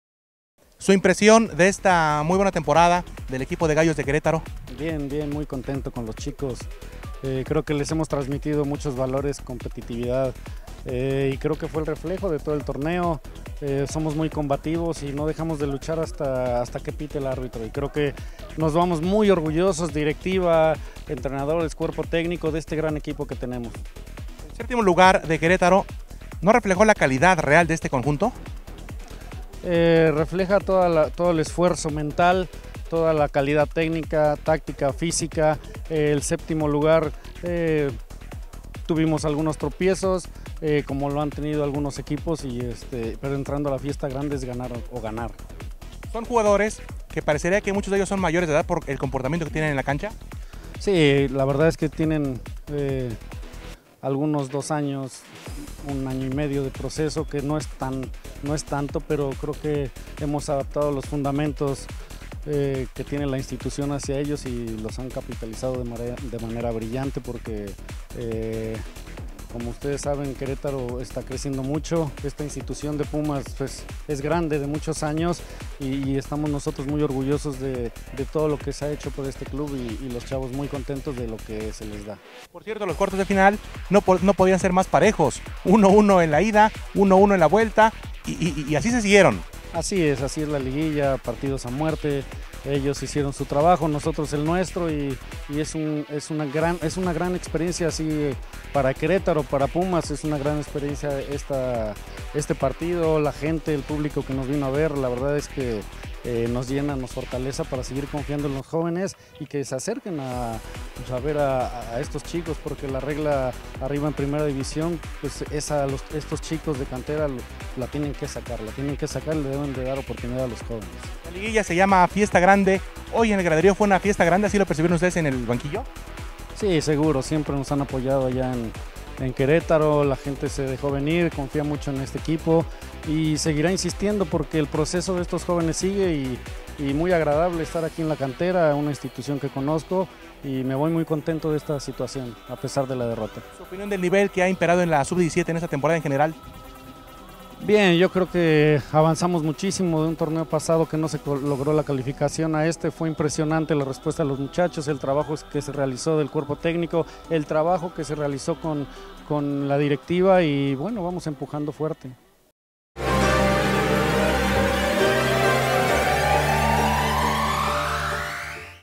Querétaro Sub 17 se Despide del Clausura 2017 con la Cara en Alto
Más allá del séptimo lugar obtenido en la temporada regular y de ser eliminados por Pumas en Cuartos de Final, los Gallos cumplieron con una buena temporada, donde los valores de la institución se respetaron en todas las canchas.

Así lo estableció el estratega Arturo Ortega, quien dirigió a la UNAM Sub 17 y Segunda División Premier, del 2011 al 2014. Nuestro entrevistado apuntó que su escuadra se va en una contienda muy pareja ante los Pumas.

Pumas y Querétaro empataron a un gol en el partido correspondiente a los Cuartos de Final de vuelta de la LIGA Bancomer MX Sub-17.

El marcador global quedó igualado a dos, la mejor posición de Pumas en la tabla general, propició el pase capitalino.

Reportero: César Santiesteban